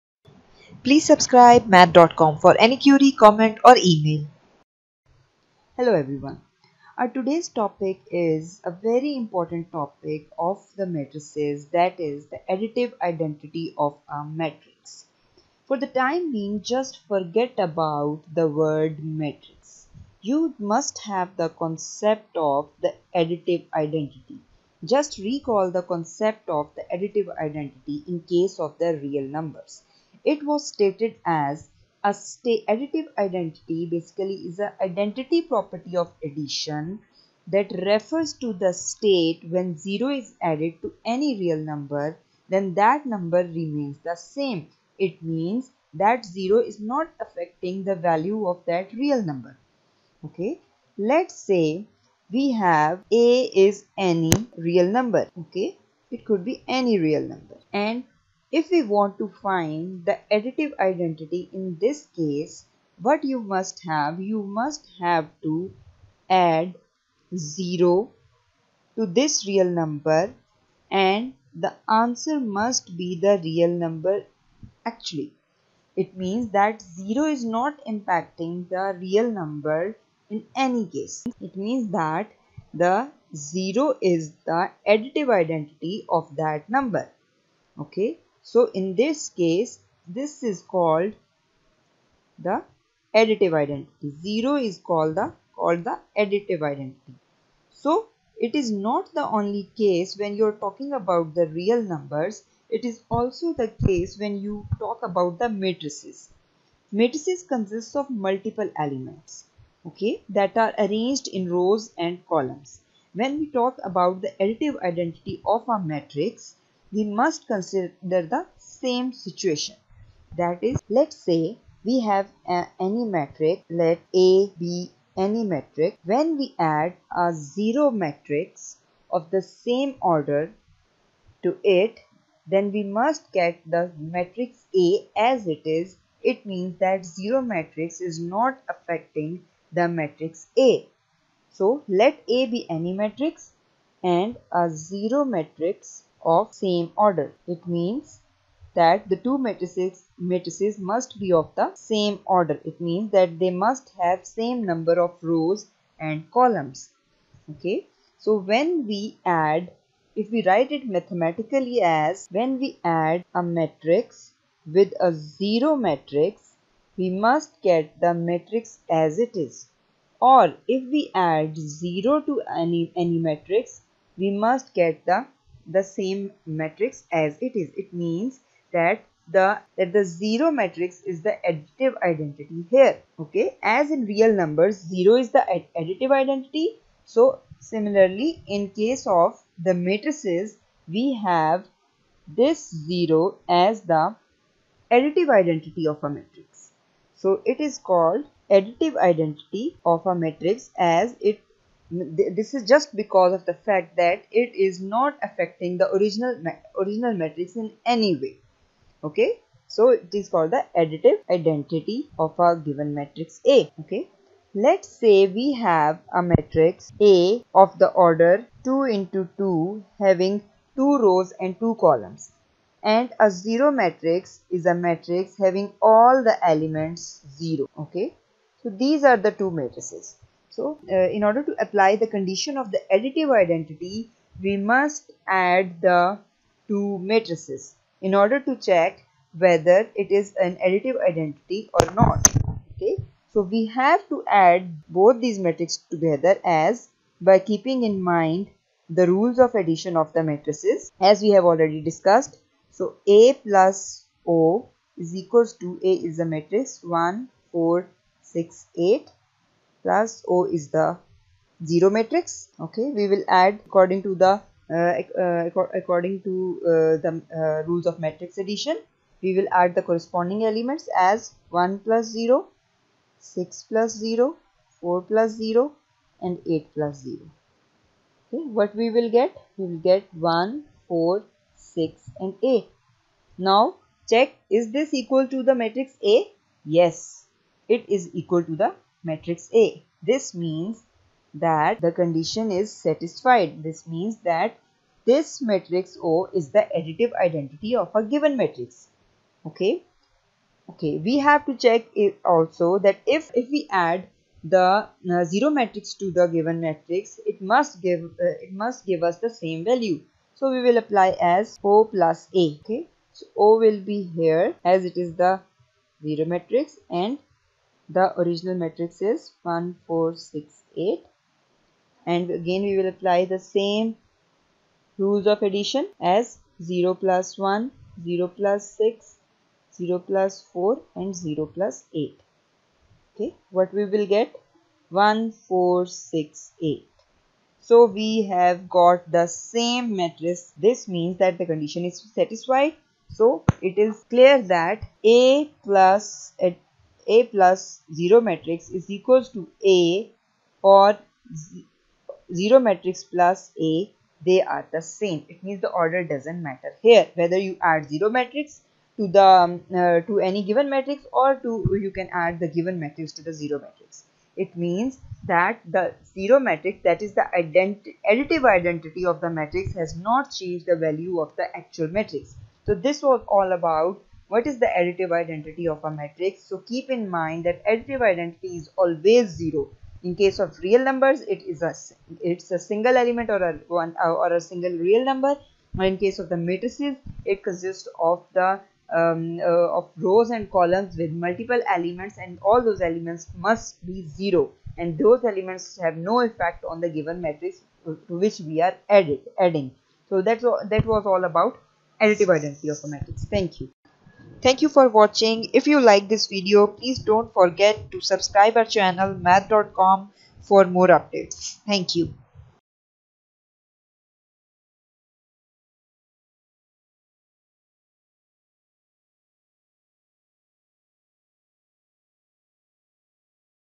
What is Additive Identity | Additive Identity of a Matrix | Matrices and Determinants | Math Dot Com
Identity Property of Addition refers to the state when zero is added to any real numbers or matrices and the real number or matrices remain same.Additive Identity is zero for real numbers as well as matrices.For any query related to additive identity of a Matrix please comment or email at